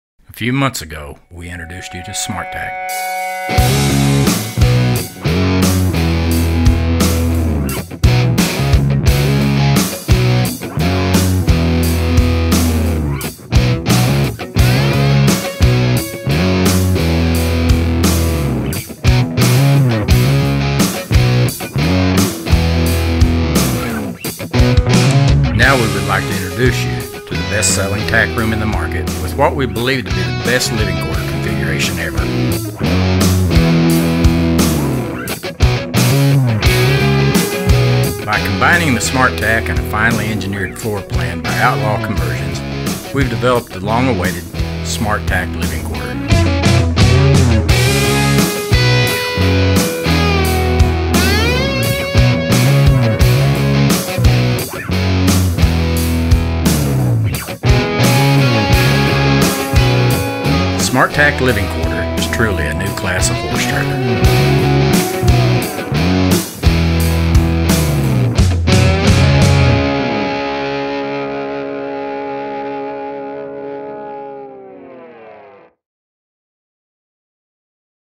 Smart Tack LQ
At the end of the day it is all about you and your horses. They say history repeats itself. For the past 48 years we have had your back around every corner and through every twist and turn. We've provided you with safety, reliability, trust, comfort, convenience, customization, and longevity. The all new Smart Tack LQ is history with a twist. Everything you have come to expect from your Hart trailer over the last half a century with a little something extra. Convenience and innovation marry at the highest level to give you the ultimate in life on the road. We've teamed up with Outlaw Conversions and we introduce to you the very first trailer of it's kind. The Smart Tack LQ. Everything you need. In one trailer.